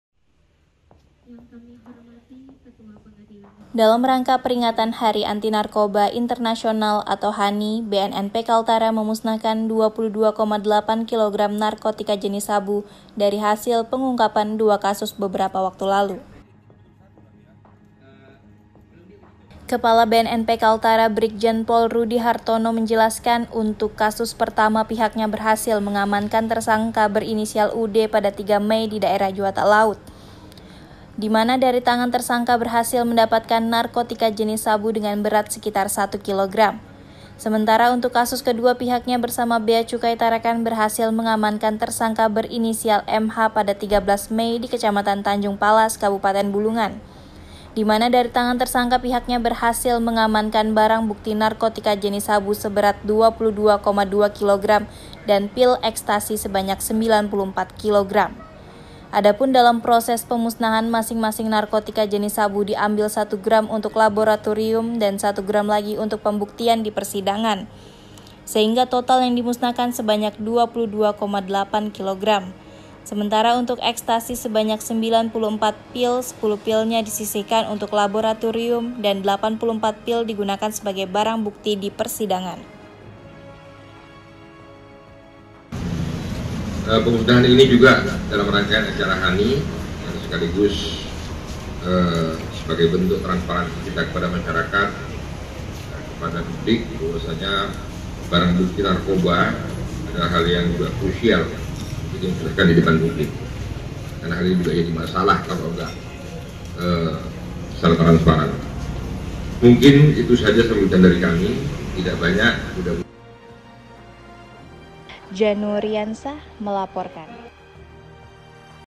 BNNP KALTARA MUSNAHKAN 22,8 KG SABU HASIL PENGUNGKAPAN SEBULAN
PEMUSNAHAN DALAM PERINGATAN HANI

TARAKAN
DALAM RANGKA PERINGATAN HARI ANTI NARKOTIKA INTERNASIONAL ATAU HANI, BNNP KALTARA MEMUSNAHKAN 22,8 KG NARKOTIKA JENIS SABU DARI HASIL PENGUNGKAPAN 2 KASUS BEBERAPA WAKTU LALU.